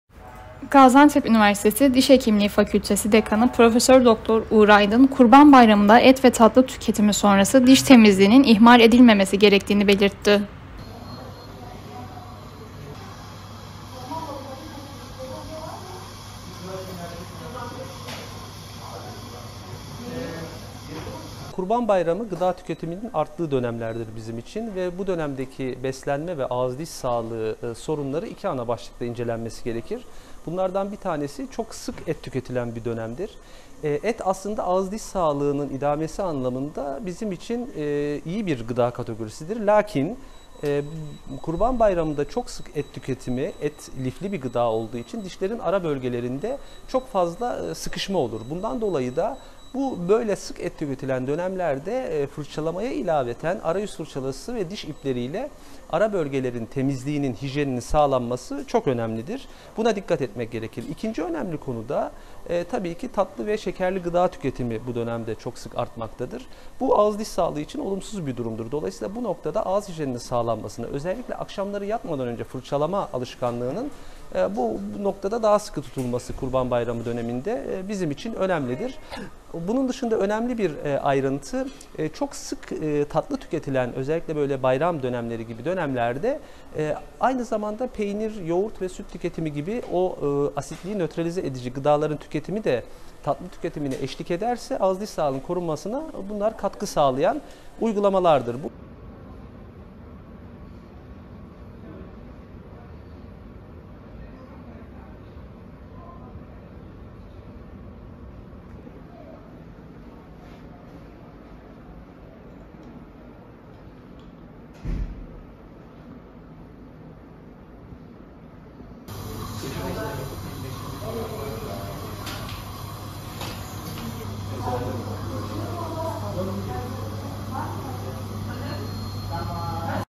Kurban Bayramı'nda Ağız Ve Diş Sağlığı Bakımı Uyarısı
Gaziantep Üniversitesi Diş Hekimliği Fakültesi Dekanı Prof. Dr. Uğur Aydın, Kurban Bayramı'nda, et ve tatlı tüketimi sonrası diş temizliğinin ihmal edilmemesi gerektiğini belirtti.